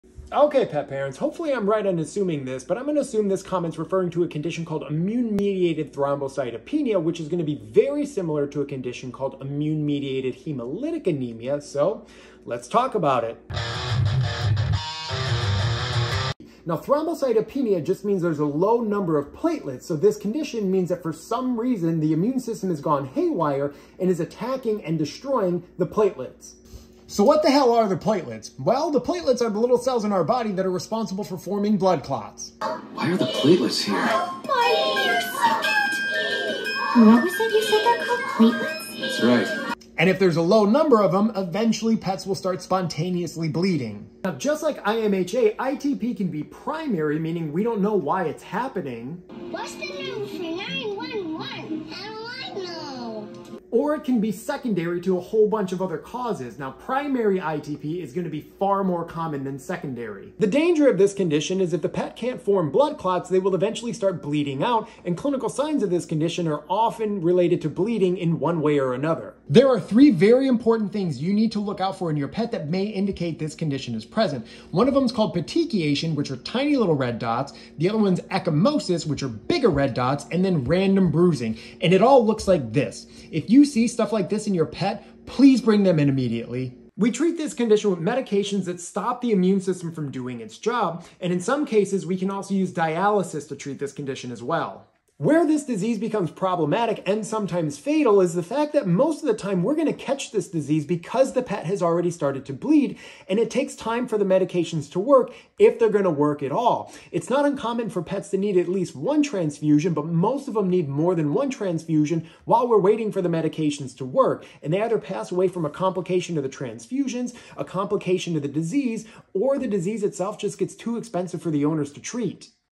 Immune Mediated Thrombocytopenia (ITP or IMT)
The immune system is a weird thing. It's there to protect the body, but sometimes it turns on the friendly fire and starts attacking the body it is intended to protect. The immune system can also attack any part of the body it wants with no true rhyme or reason.

With this condition, the immune system has decided to attack the platelets, which are the cells that are responsible for forming blood clots. If the platelet counts gets below a certain level, the pet will be at risk for spontaneous bleeding. This means the pet can randomly start bleeding out for no reason. The random bruising and red spots discussed in the video are a pretty concerning sign to see in your pet. They can be there from other conditions, but regardless of why you see the bruising or red spots, it is a big concern.